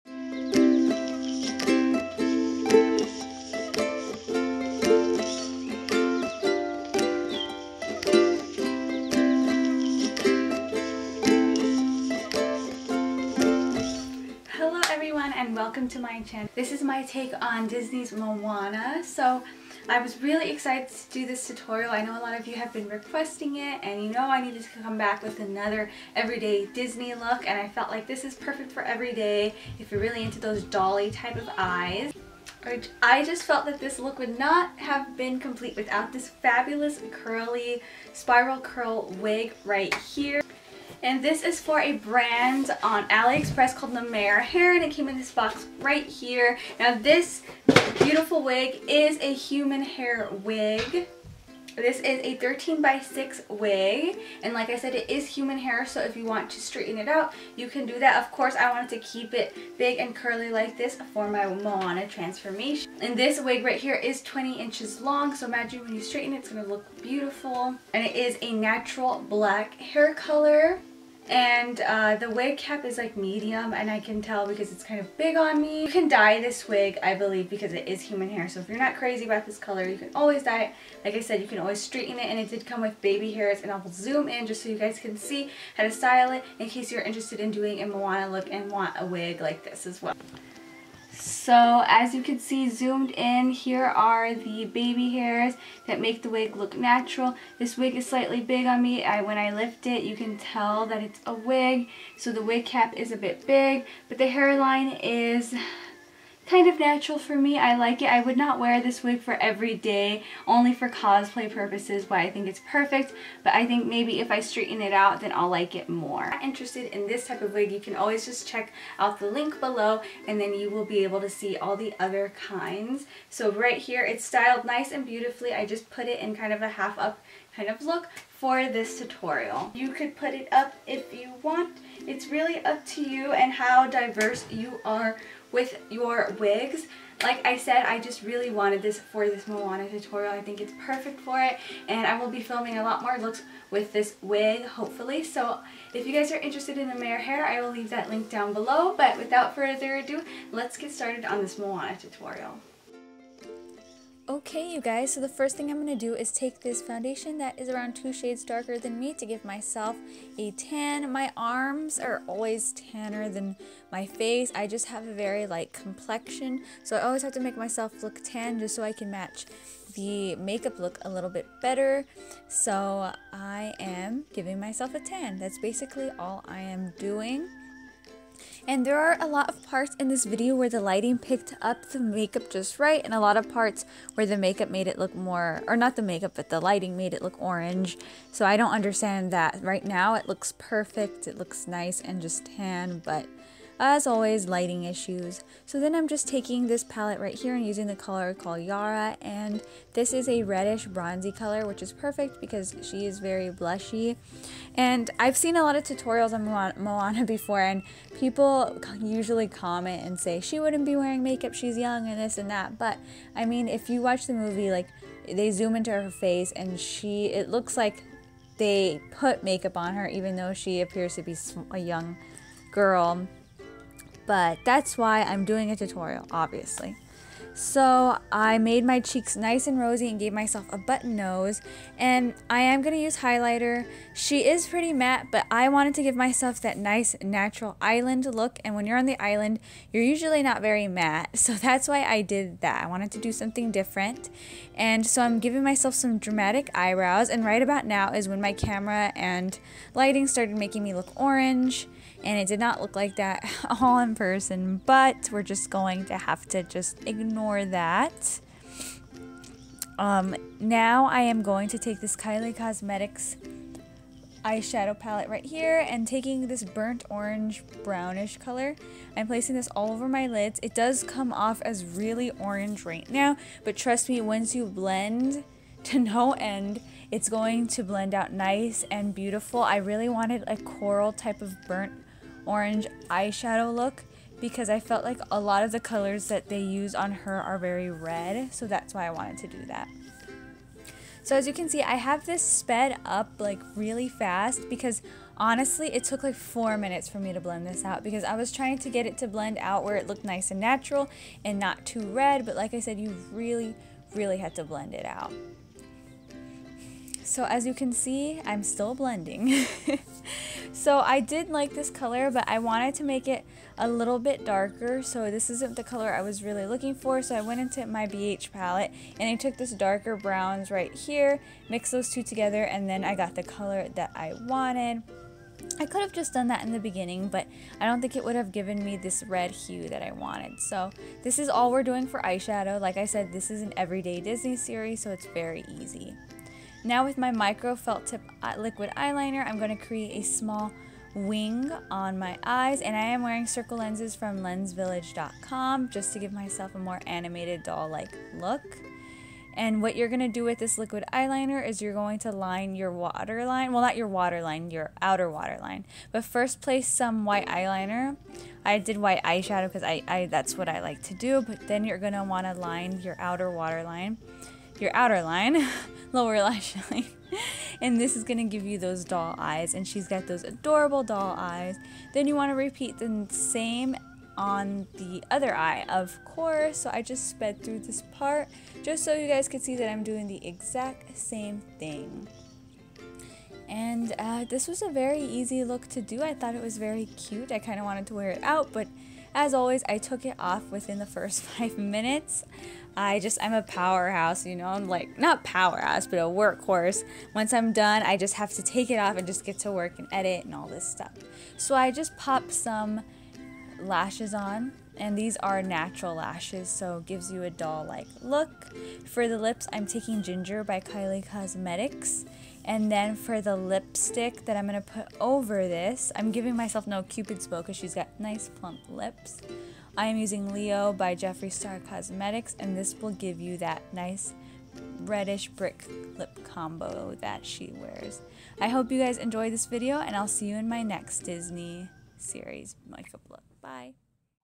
Everyday Disney Series: Moana Makeup feat. Nemer Hair
hello loves! ❤ i am so excited to present to you my take on Disney's Moana! i had a lot of fun creating this look for you guys. as always i try to make it unique & wearable for everyday.

WIG DETAILS

Item details:
Style: curly hair 13x6 lace front human hair wig
Length: 20 inch
Texture: curly
Color: natural black
Density: 150%
Cap: medium brown & medium size